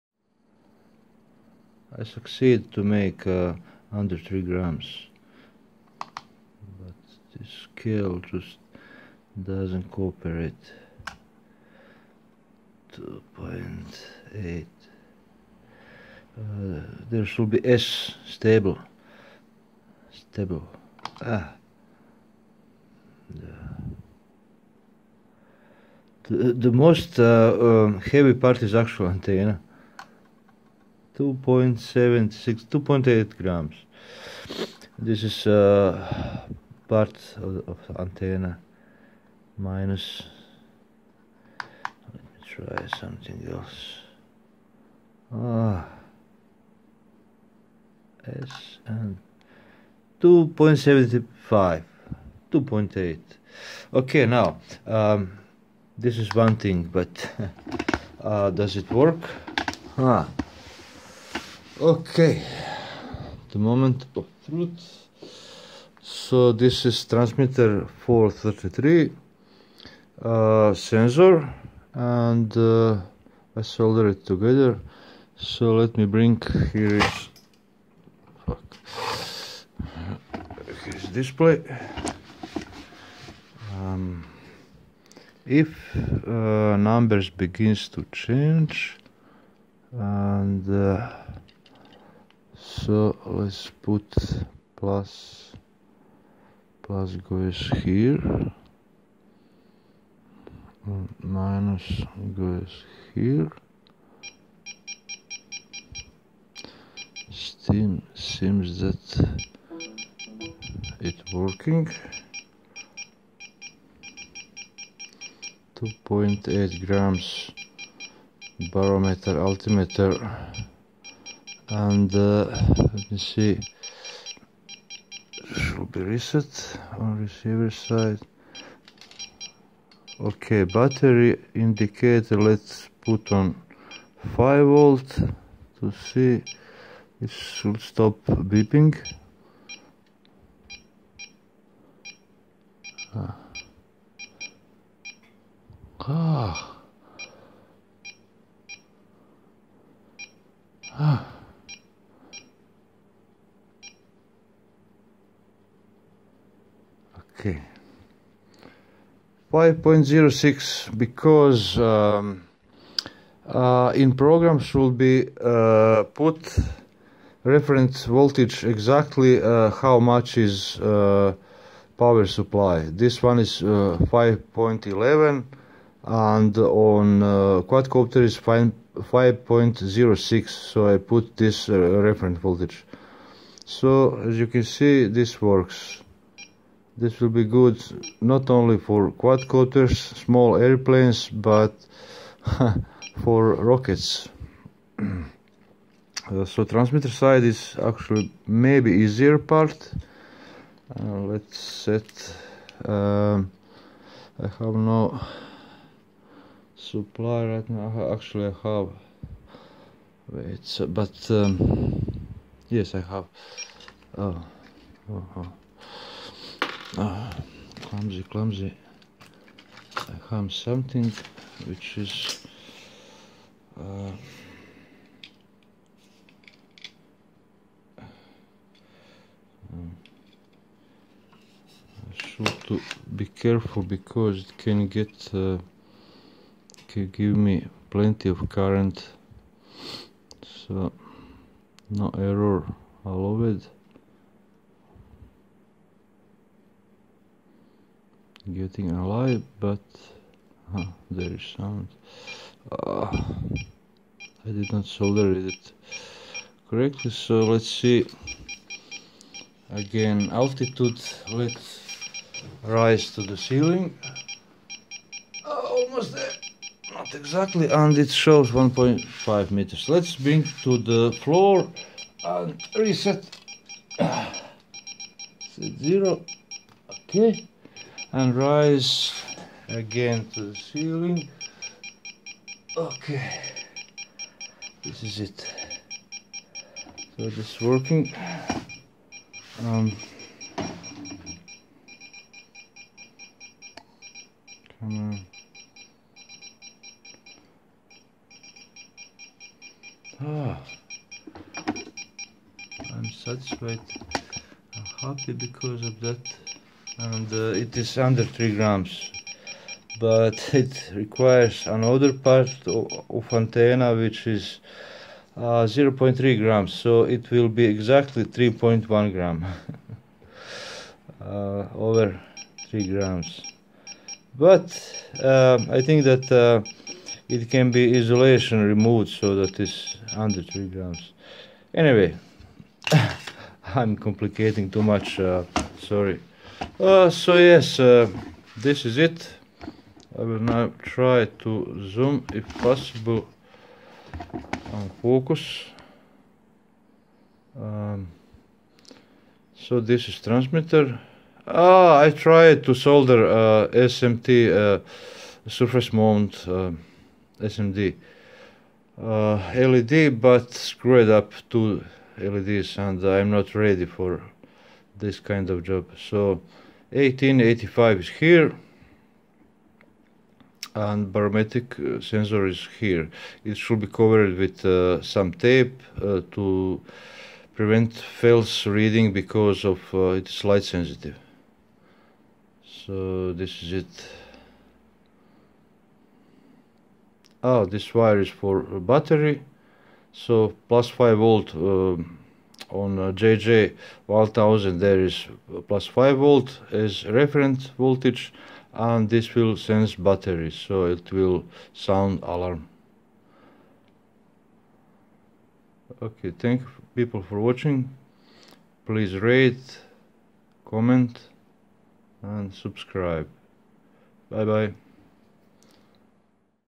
Arduino Altimeter Under 3 Grams
Finally got some decent sketch for Attiny85. It is not a problem to get it working, but the problem is with me - I want more features, as is pressure, temperature, ID of the vehicle, battery voltage... just... if I put everything, it will be pretty slow. Also, still waiting some LCD and TFT displays to arrive. Currently, this small OLED display is very limited by size and amount of text that can show. So, I gave up of temperature reading (on the quadcopter), no current sensing, and other things that can be theoretically sent to the base station as telemetry. This is first version which will rely on good reception in order to showing current and maximum reached altitude. In plan is another altimeter, for which I am not sure whether I will be able to find lightweight LCD or some other display. So, it can work without any display, but require EEPROM memory and some base station for transfer recorded data. Since I am not yet in rocketry, this will be done much later, but idea is intriguing...
Testing, I mean real testing will be done when base station will be finished. There is some problem with 'wild numbers', which sometimes appear, and I am not yet sure is it on transmitter end, or receiver end. Everything telling me that this is due to OLED 'virtual' memory - whole screen is memorized on arduino board (S-RAM), then after writing and drawing, it is send to display. Other displays as is 1602 or my favorite Nextion (waiting, waiting...) has its own screen memory, so it will be pleasure to work with. This high memory 'consumption' may lead to some instability, and I am not sue how to debug it. Aside that, OLED is not so bright, and on daytime hard to see (aside too small letters).